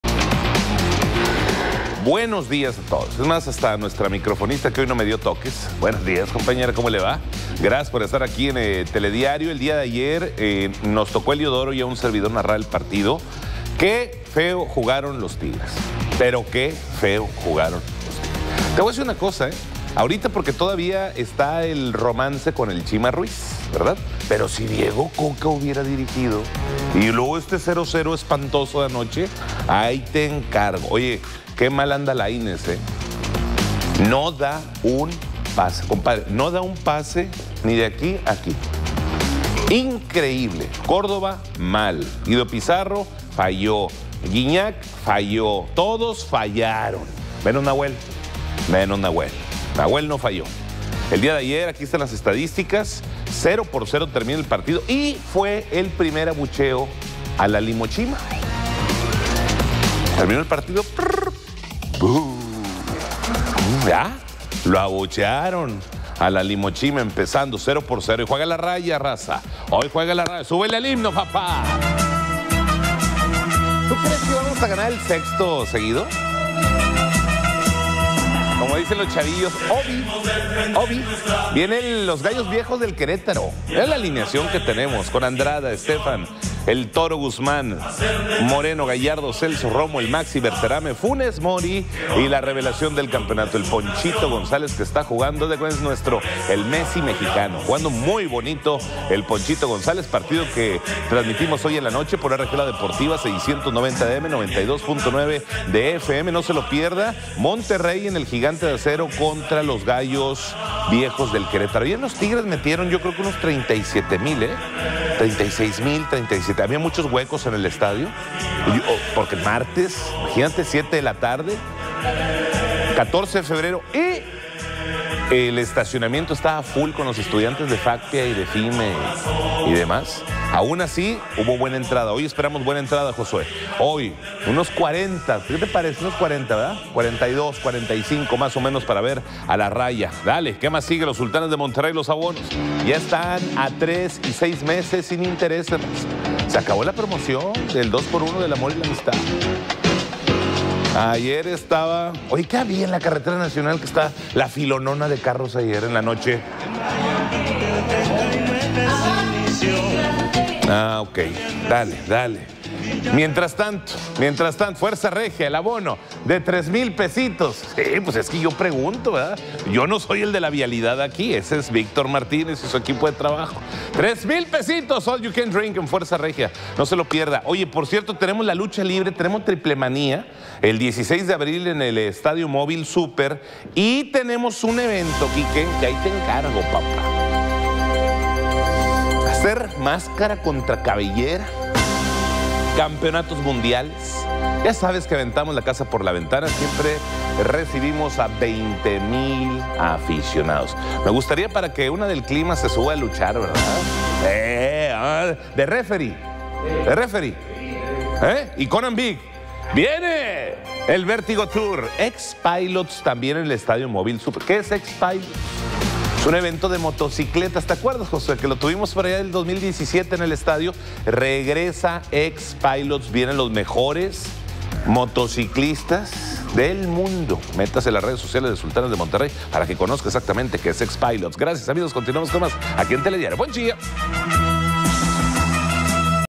Qué feo jugó Tigres; Diego Lainez no da un pase bien
Qué feo jugó Tigres. Diego Lainez no da un pase bien. Todos fallaron. Menos Nahuel. Hubo el primer abucheo a la LimoChima.